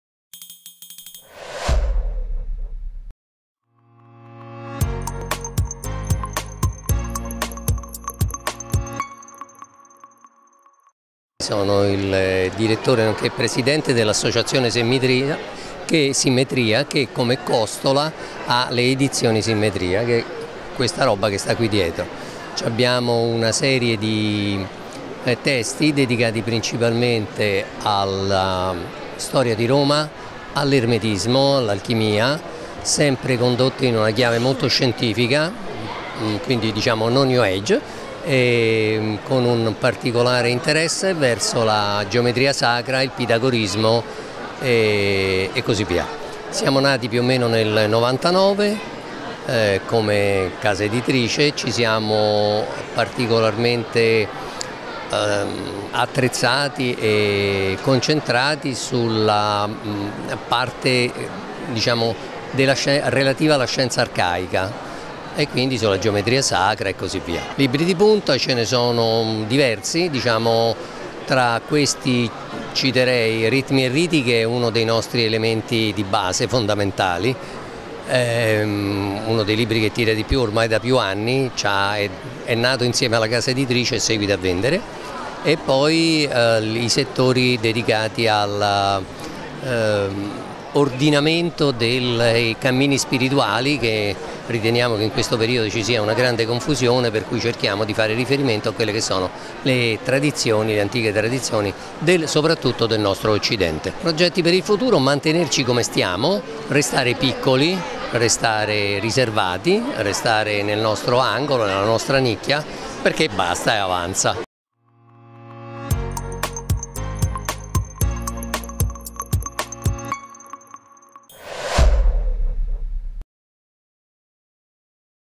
Claudio LANZI (Simmetria Edizioni). Più libri più liberi, edizione 2017.
Dal 6 al 10 dicembre 2017 ha avuto luogo, presso il Roma Convention Center La Nuvola, la sedicesima edizione di "Più libri più liberi", la Fiera Nazionale della Piccola e Media Editoria, promossa e organizzata dall’Associazione Italiana Editori (AIE).
Tra gli editori presenti, provenienti da tutta Italia, c'era anche Simmetria Edizioni, rappresentata da Claudio LANZI.